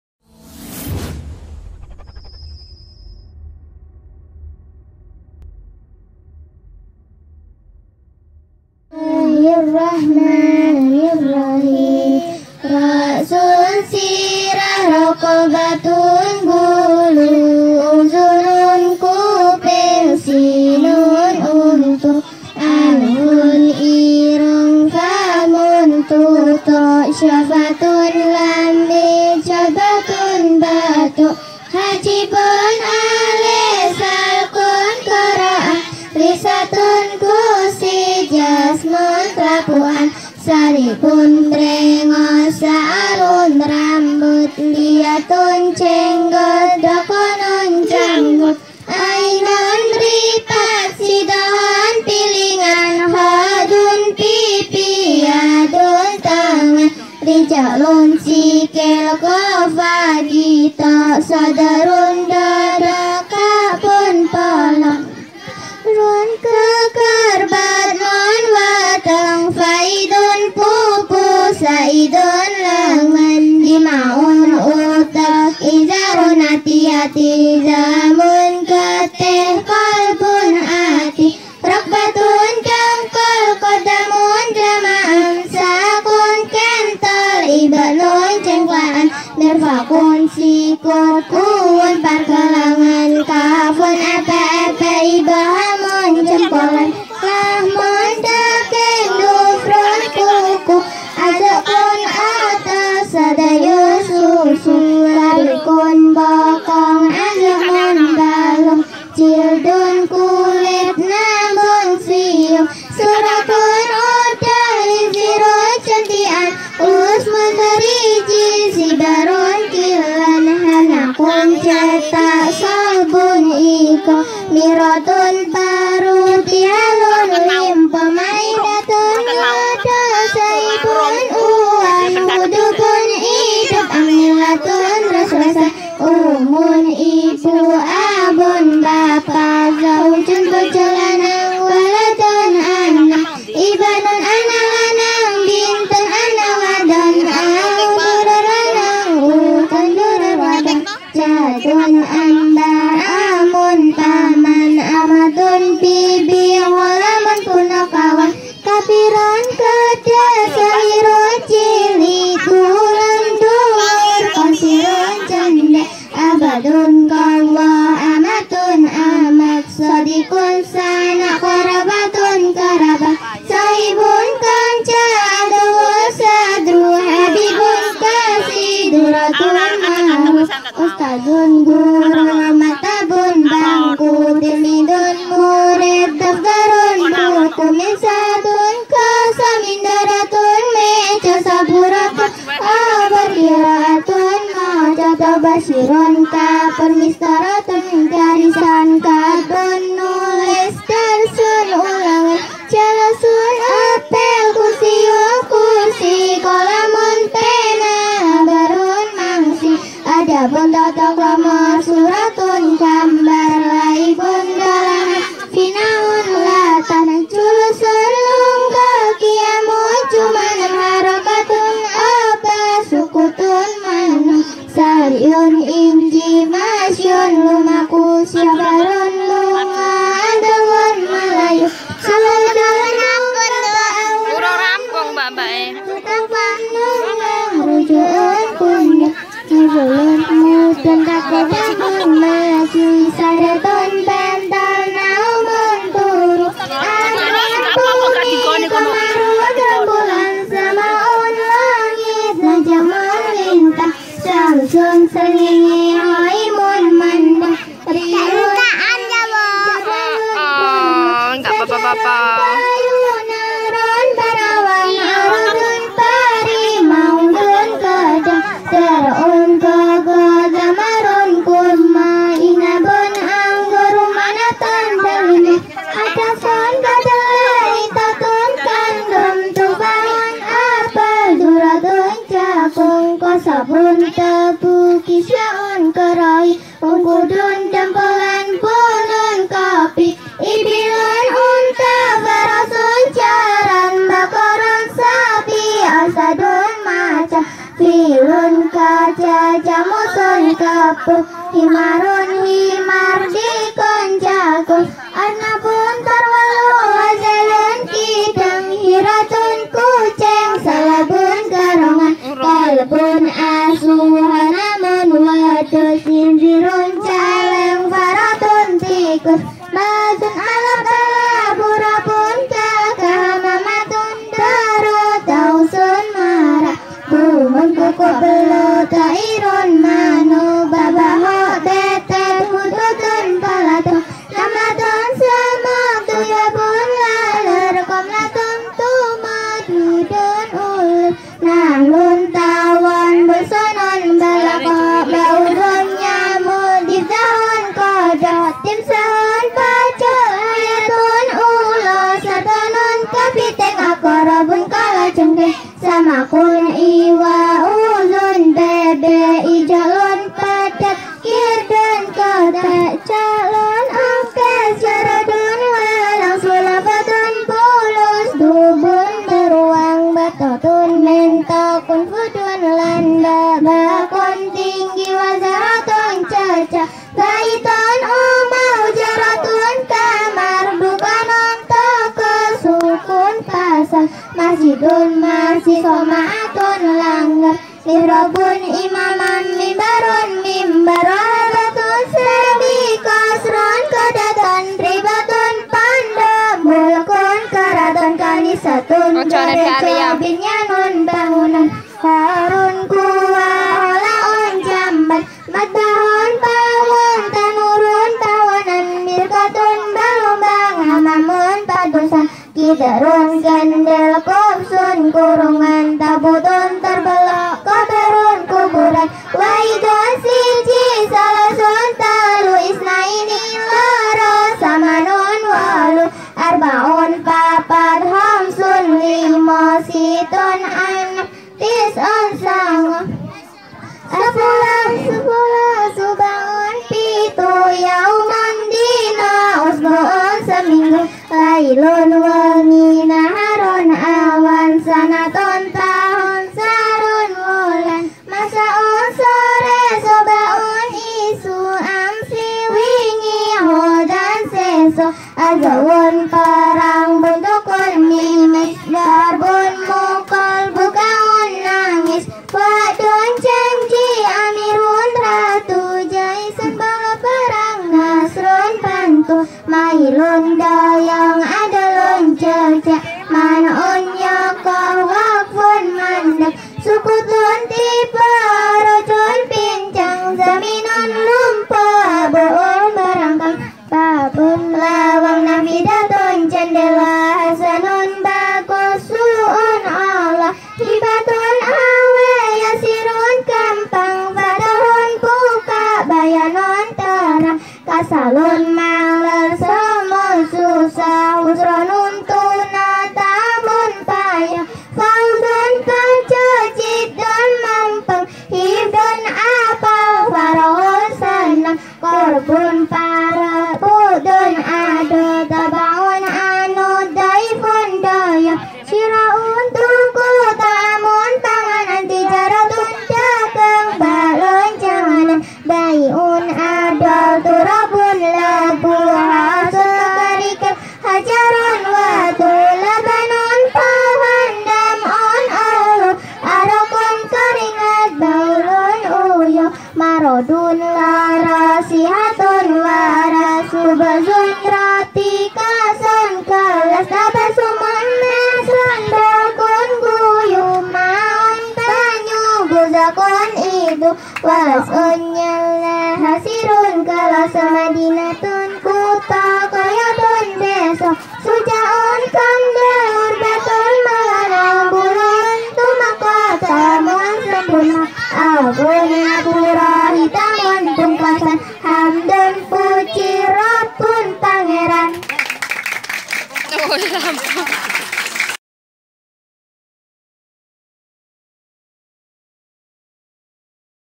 Hafidzoh kitab ro'sun sirah (Perjuangan dari desa)
Hafidzoh kitab ro'sun sirah di TPA terpencil, Tanpa gedung khsusu untuk mengajar, ibu ustadzah ini terus mengajarkan ilmunya kepada anak-anak di desa terpencil, tetap semangat, dan bahkan anak didiknya ada yang hafidz kitab roksun sirah, dan ada juga yang khatam kitab alala.